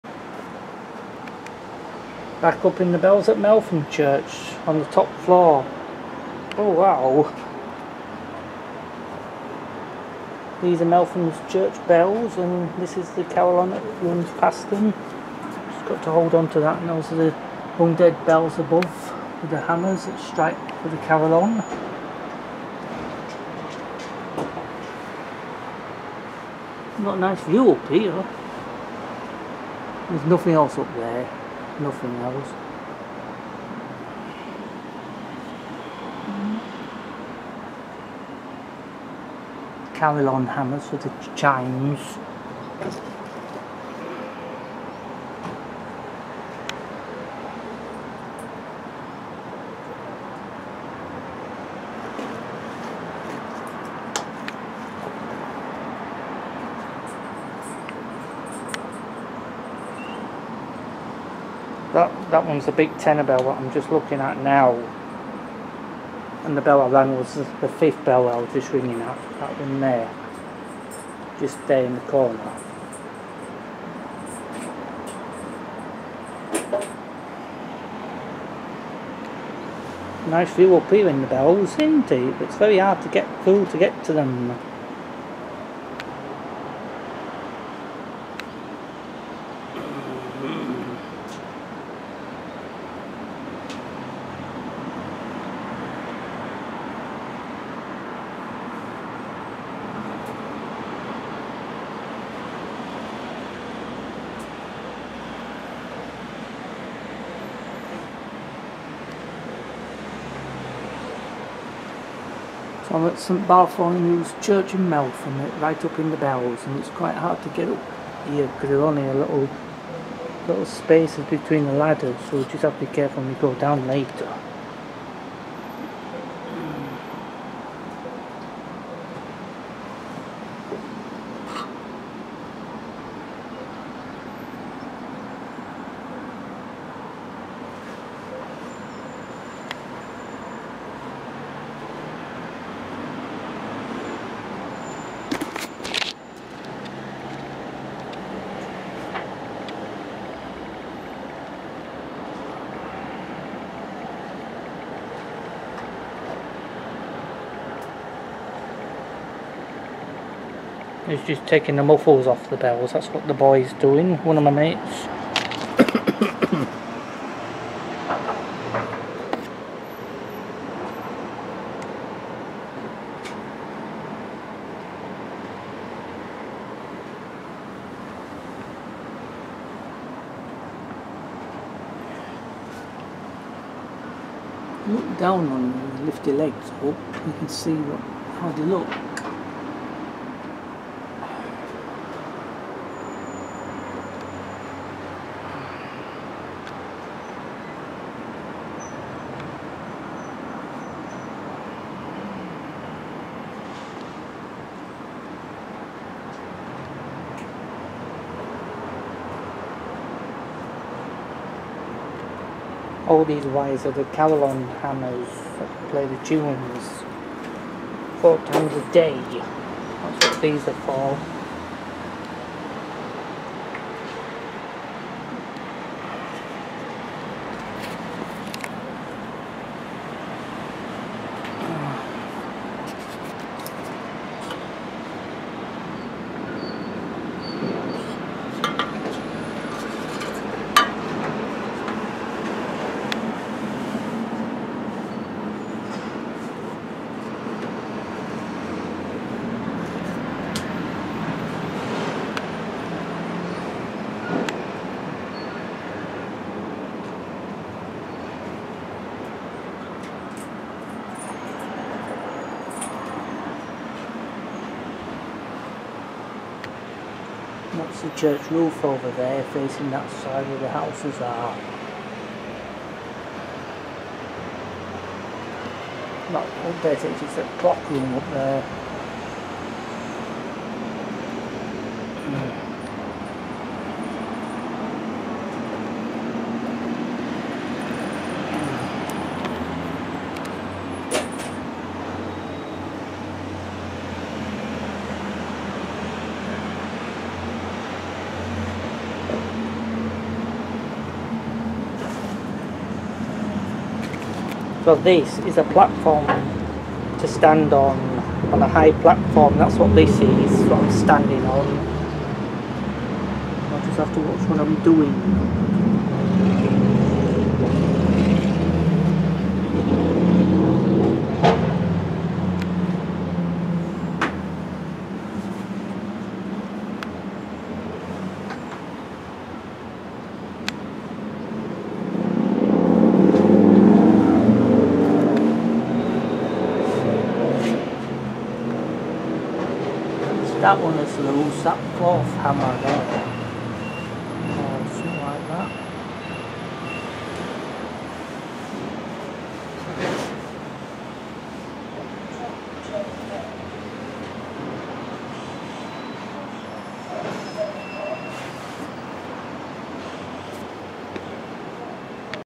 thursday th24 october 2024 meltham church bells on the high up floor
yes just above the bells because they have been refurbished and look better with the red head stocks on the bells i did this after the funeral had finished and when we both took the muffles of the clappers better going up and seeing the bells and never have seen them before you can see them on my youtube channel i was extreamley very very carfull going up the 2 wooden ladders no falls no grazes no all went well i dont mind putting church bells on youtube i may never be able to do that again the rules change every year health and saftey yes i didnt  stay up there when they were all upside down i came straight down with my bellringing friend to get the other 5 bells up ready for ringing no problems at all all church bells towers are diffrent stuarts tour to help with the bells